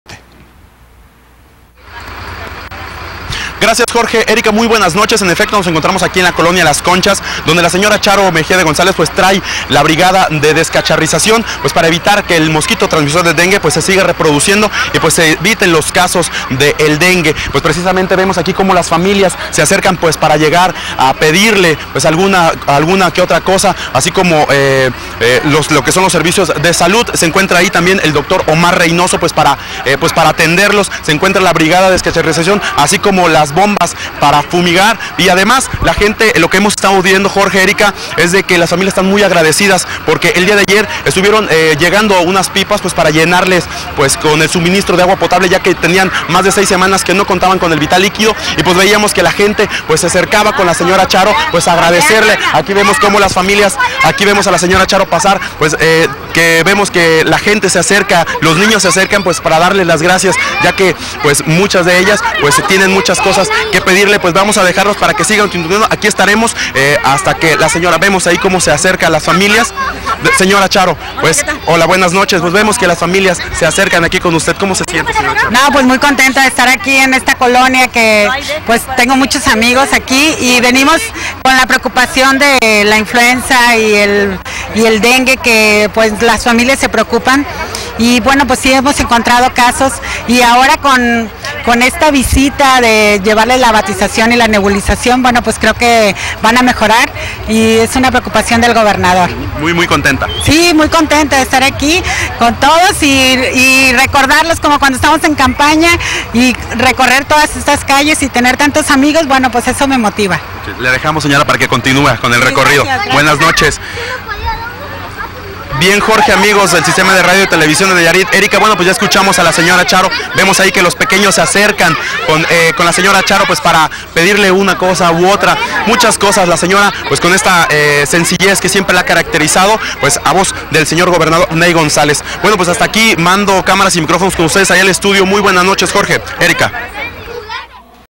SHARO EN LA COLONIA LAS CONCHAS
EN GIRA POR LA COLONIA LAS CONCHAS LA  SEÑORA SHARO MEJIA ATENDIO PERSONALMENTE LAS NECIDADES DE LAS PERSONAS QUE SE ACERCABAN A SOLICITARLE APOYOS Y SALUDARLA